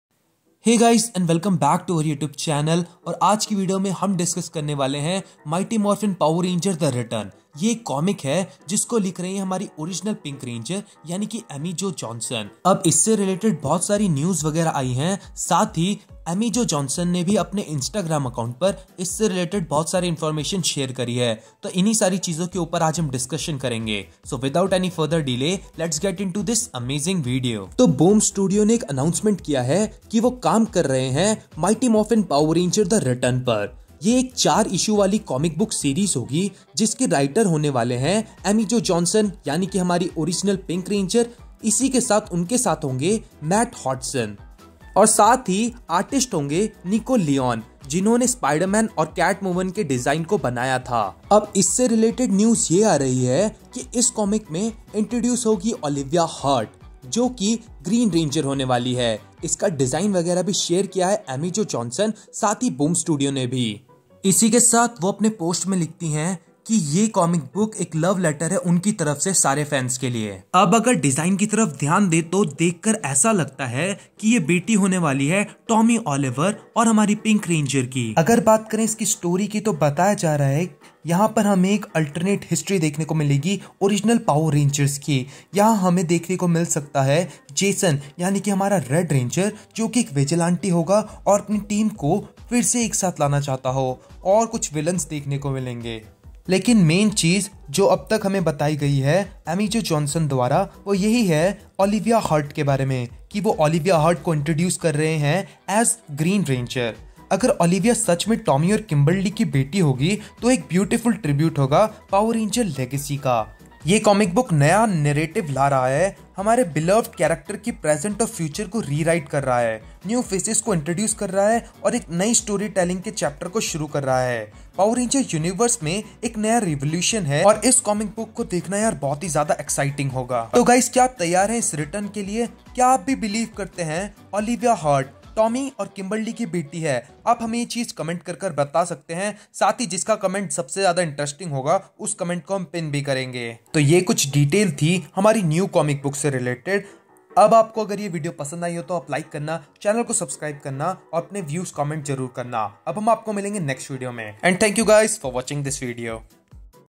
Olivia Hart Unleashed! Tommy and Kimberly| MMPR: The Return Comic News | Hindi A SQUAD
Power Rangers fans, buckle up for an epic ride as we break down the latest comic buzz - 'MMPR: The Return'! 🚀 Dive into the morphin' universe with us as we explore the intriguing storyline, meet the new Green Ranger Olivia Hart, and unravel the alternate history of the original Power Rangers. Plus, join the discussion on whether Olivia is truly the daughter of Tommy and Kimberly! Let the power protect you! ⚡️

🚀 Join the adventure now! 👉